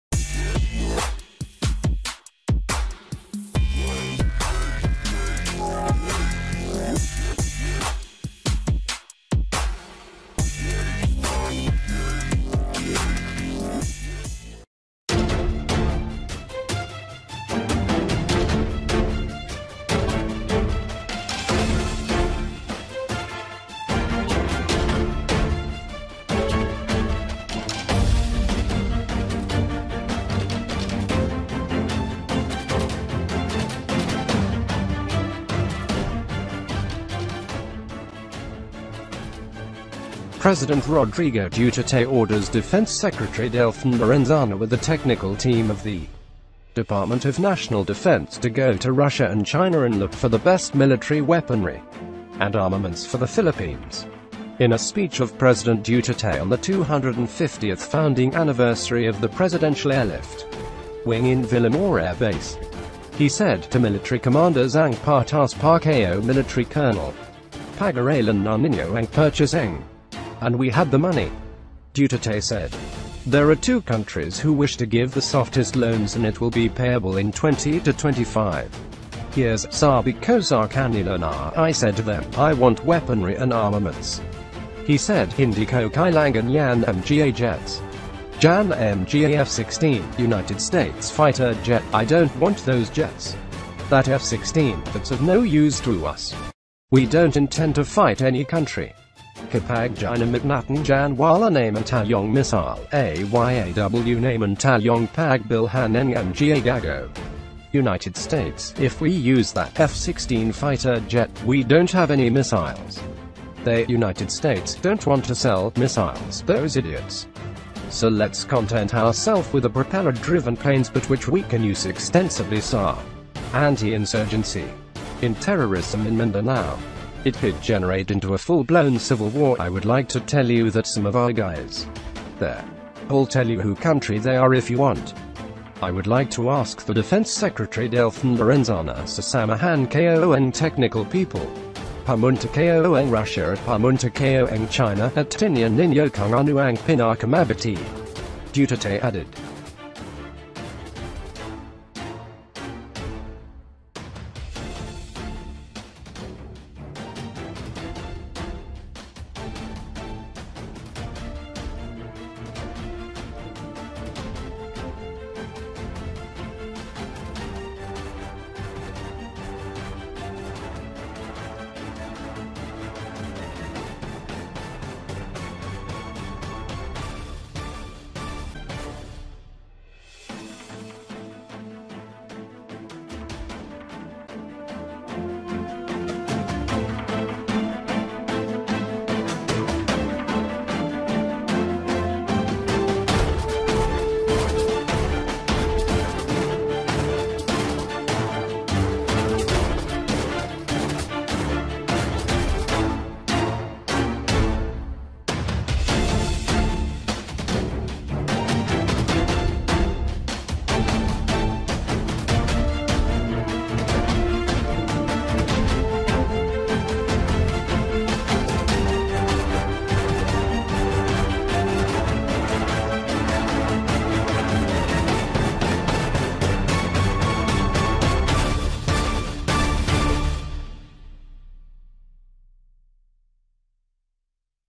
Military gear - Russia and China Best military weaponry and armaments for the Philippines
What Defense Hardware Could the Philippines Even Buy From Russia or China?